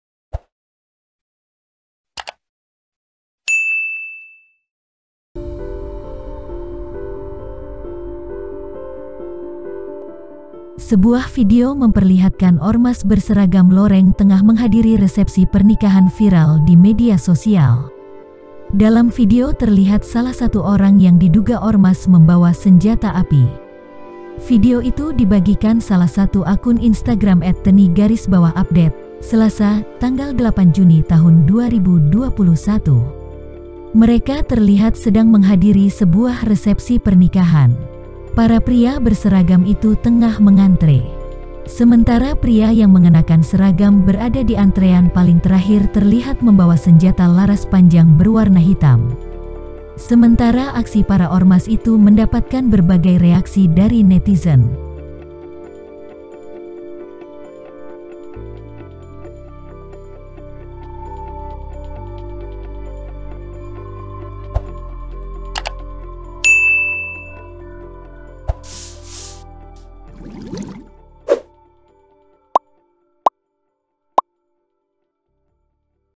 Viral, Oknum Ormas Bawa Senjata ke Kondangan
Dalam video terlihat salah satu orang yang diduga oknum ormas membawa senjata api.

Video itu dibagikan salah satu akun Instagram @tni_update, Selasa (8/6/2021).

Video terkini dan terbaru..